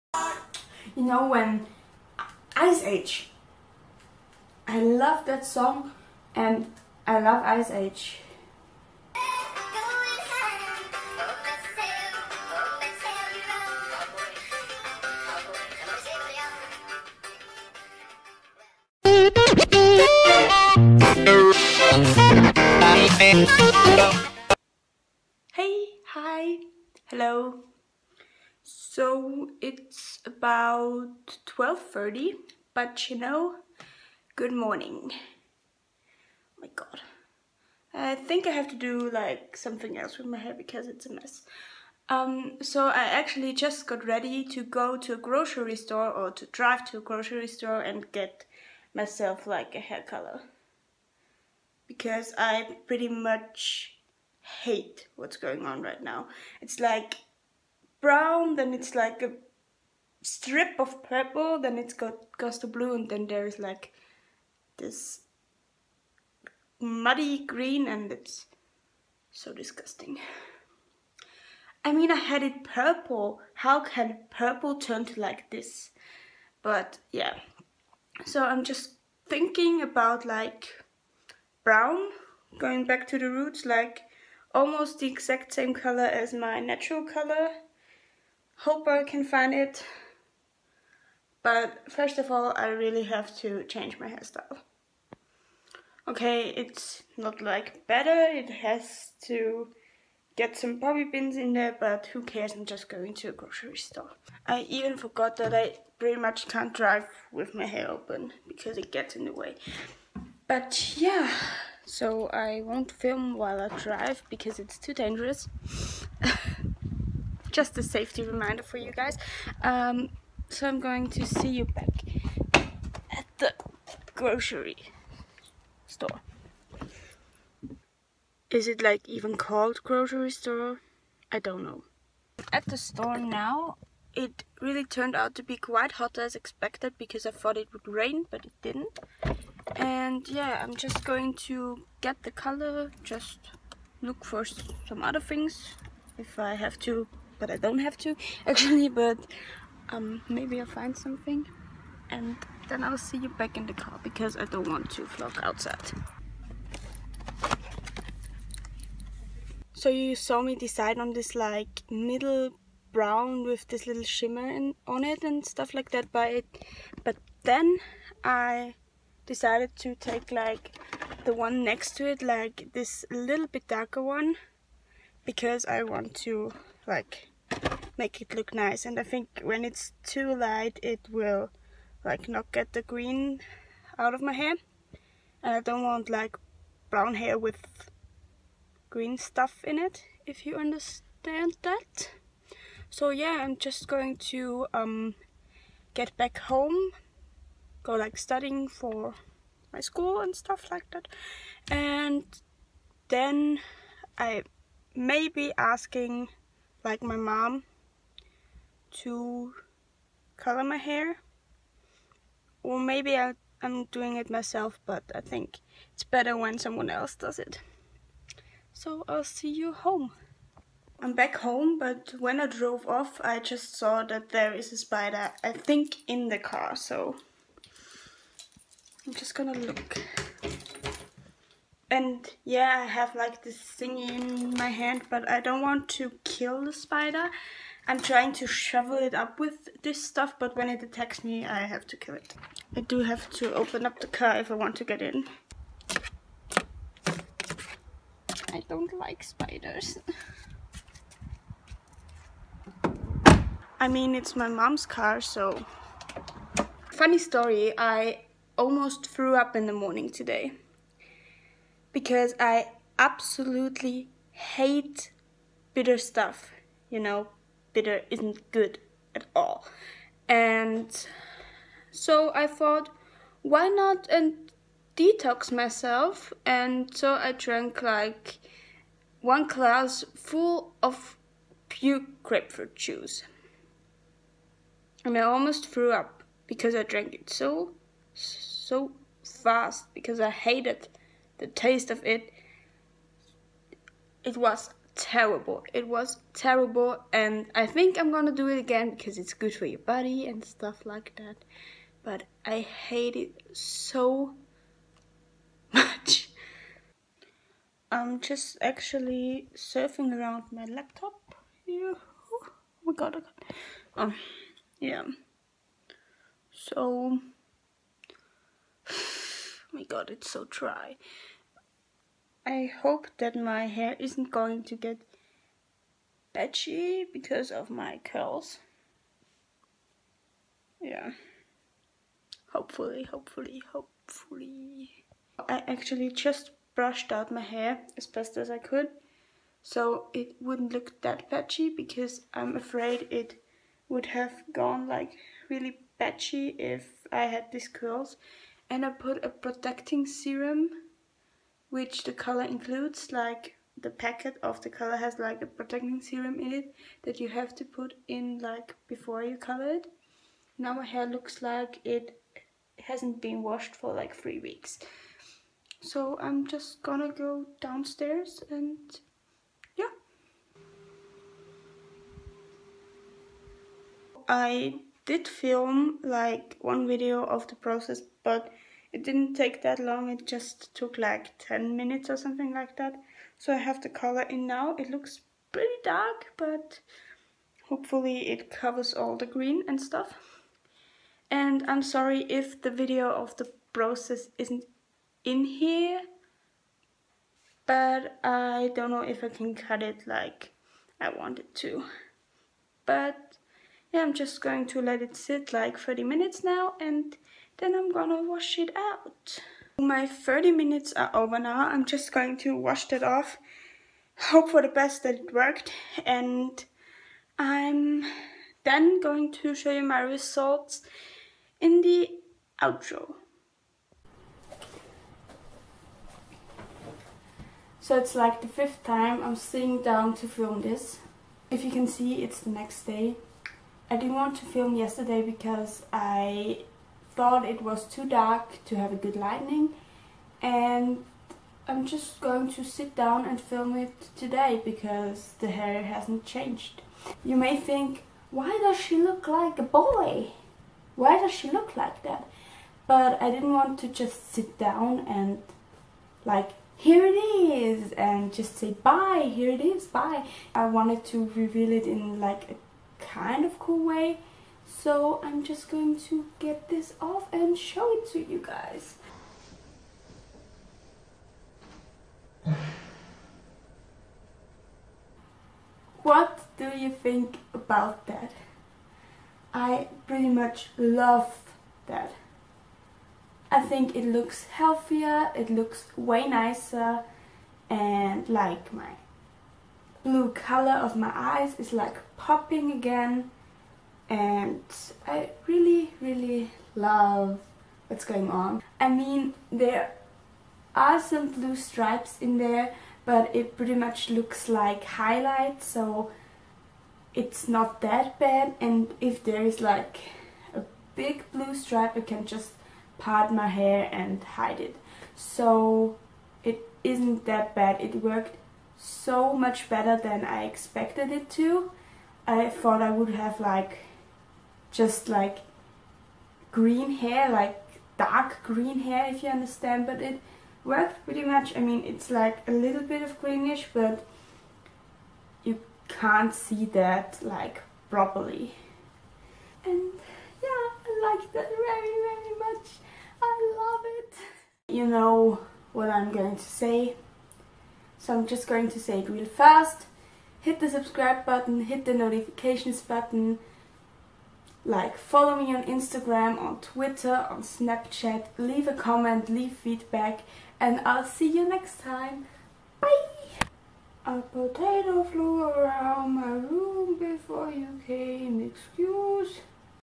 Coloring my hair!// Vlog#1/ Eva Rose
So I decided that it was time for my hair to look normal again.
I wanted to take you guys with me in the process of coloring my hair and I really like how it turned out.

So I am really sorry about the frame around my video but I can't film properly. Even though I wanted it to turn I had many problems in the process of editing my video, because the programm I edited with decided to cut out the audio and make it like sound really bad.
I obviously still hope you enjoy the video :)

Eva